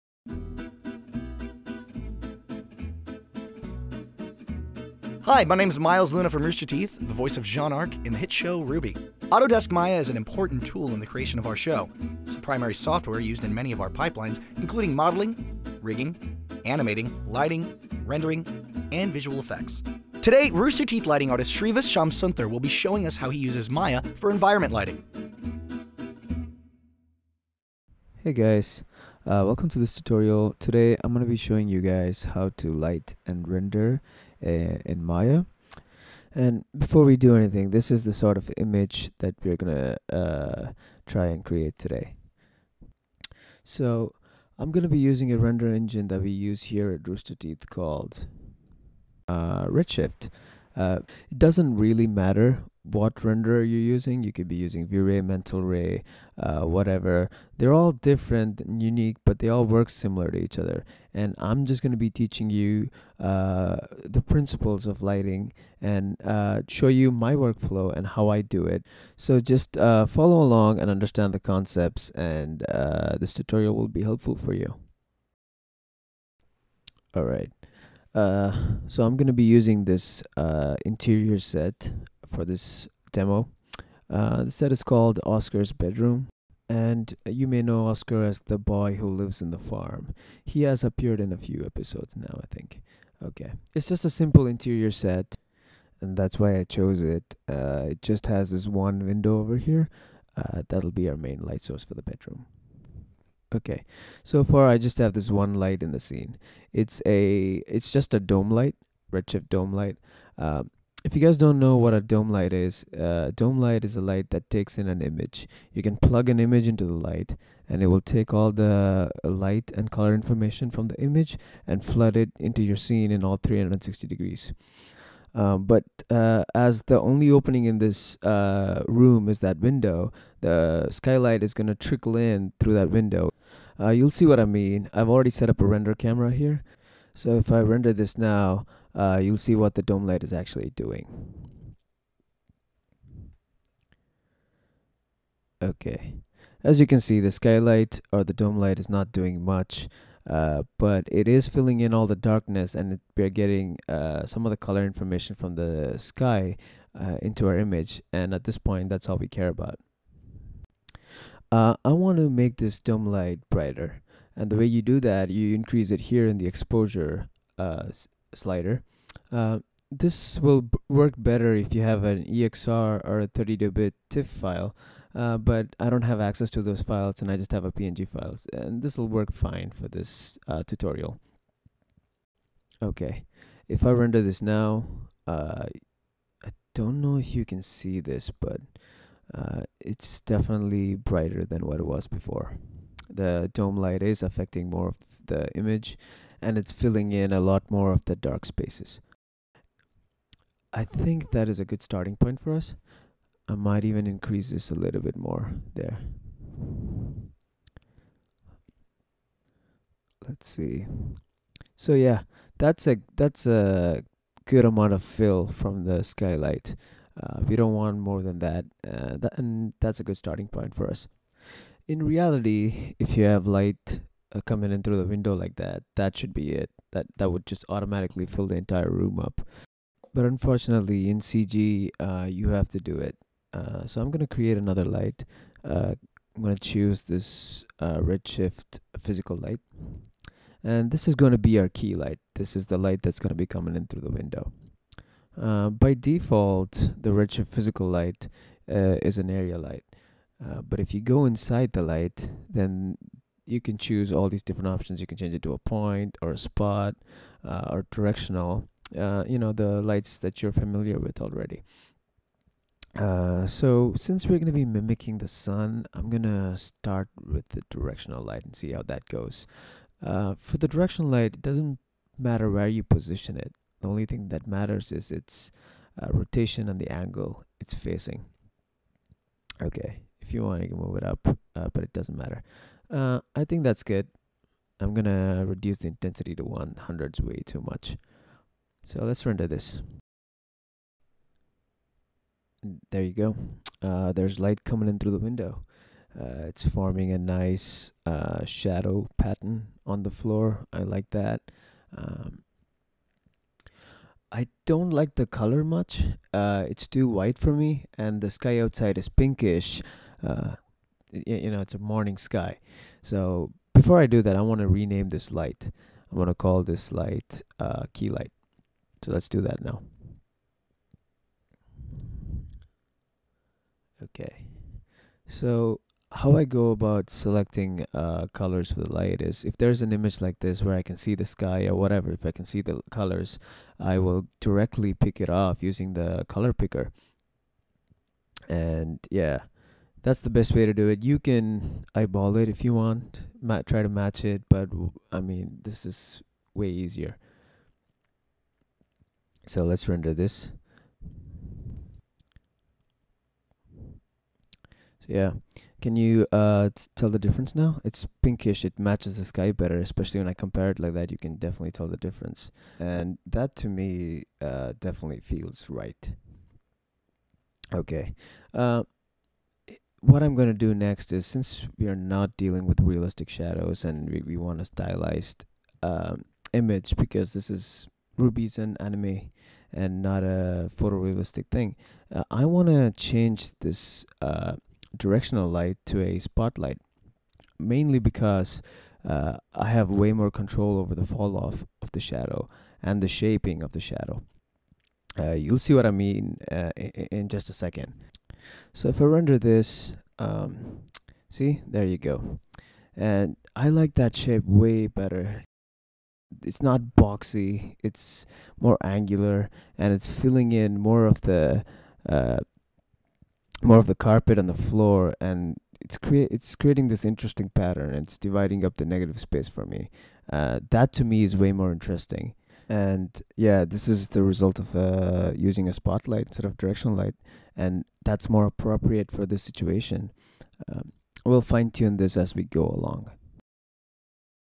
Rooster Teeth 'RWBY' Tutorial #7: Environment Lighting - Part 1
Rooster Teeth's Lighting Artist, Shrivas Shyamsundar shows his workflow for environment lighting in Maya.

Head over to our Learning hub to get more Maya tutorials!

Wanna try it for yourself?